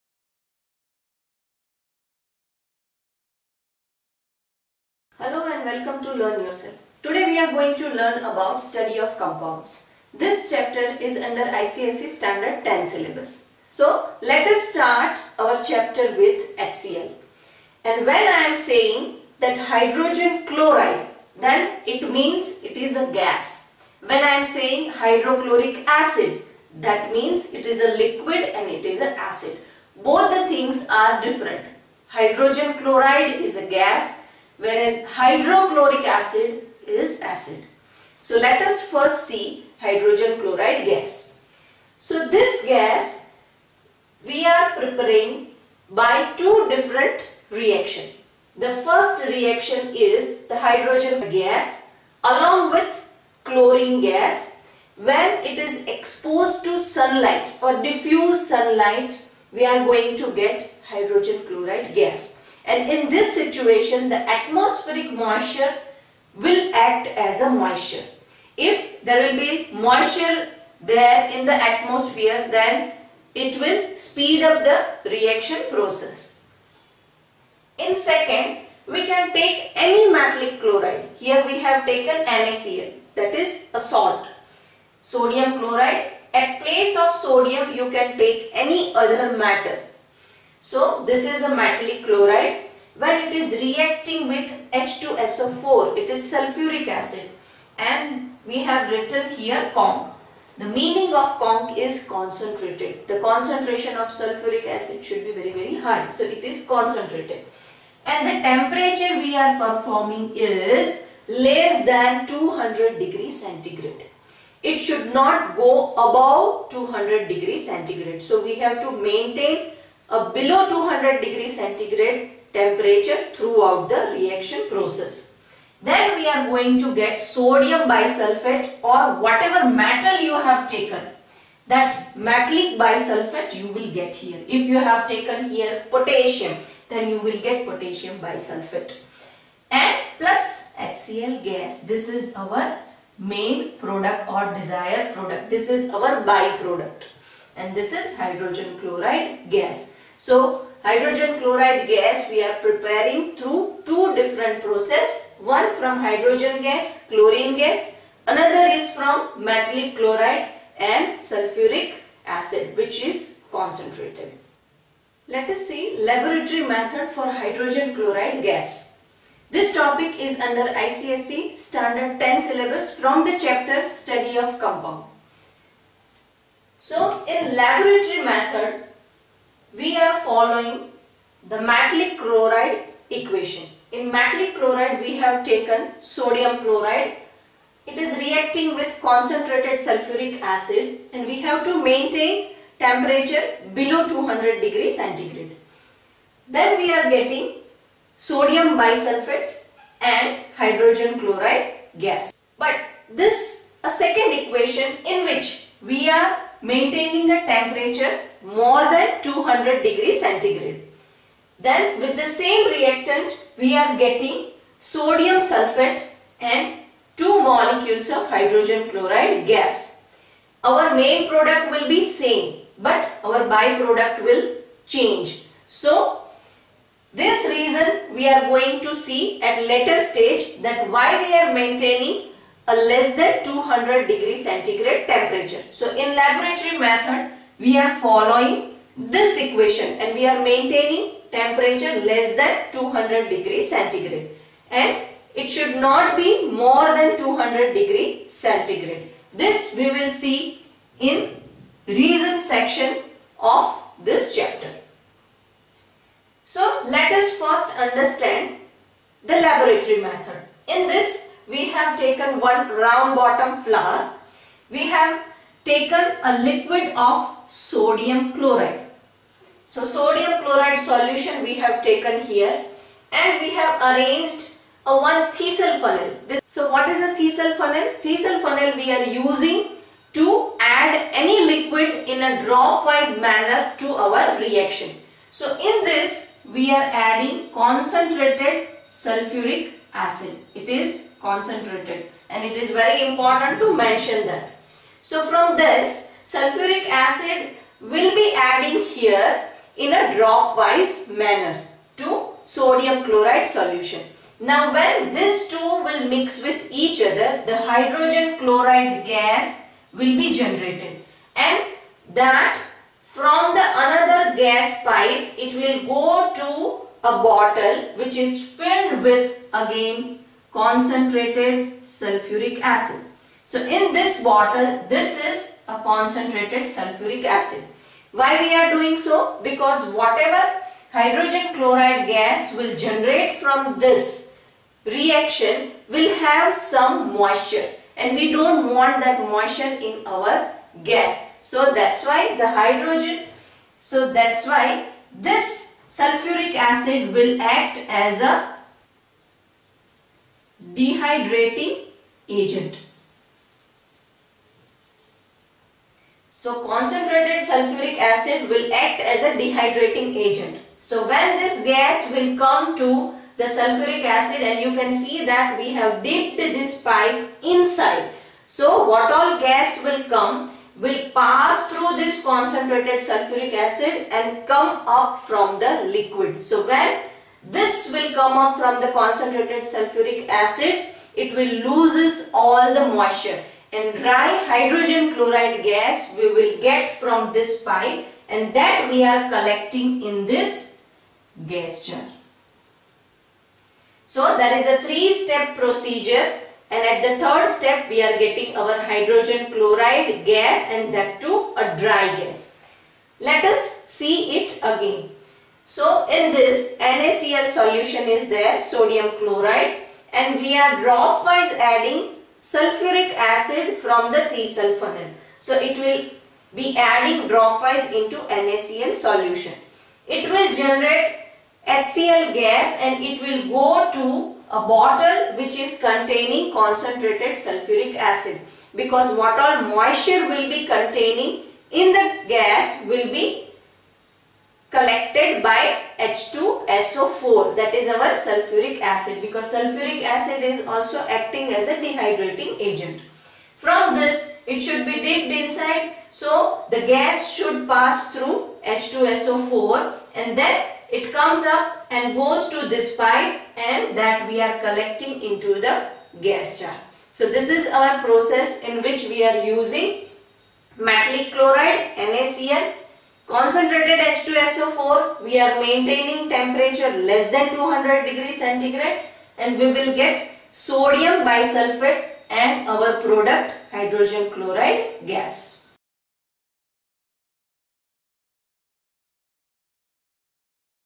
ICSE Std-10 Chemistry- Hydrogen chloride Gas Preparation via laboratory method ||Study of compounds
ICSE/Std X/Chp 7- Study of Compounds-  HCl- Lab Preparation
Chemical Reactions and Laboratory Preparations of Hydrogen chloride gas